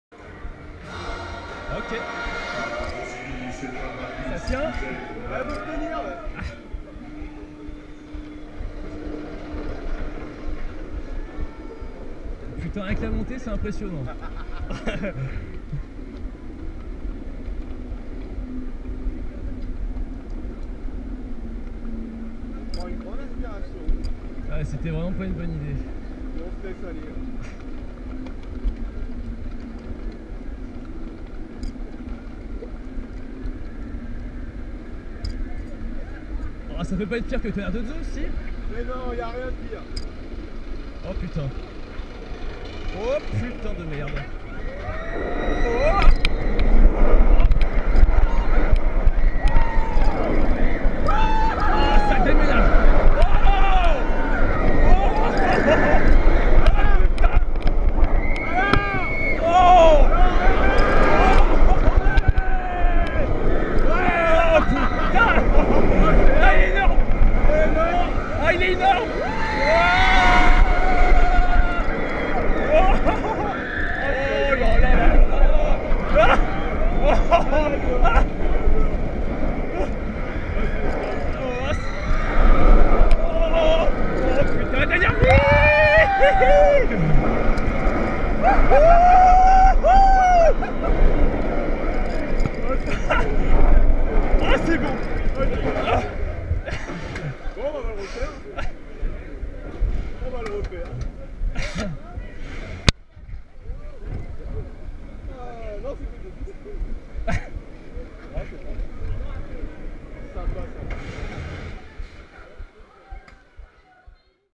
#16 (+) Parc Astérix - Oziris (onride) - GoPro 3+
Vue embarquée de l'impressionnant Oziris, attraction phare du Parc Astérix !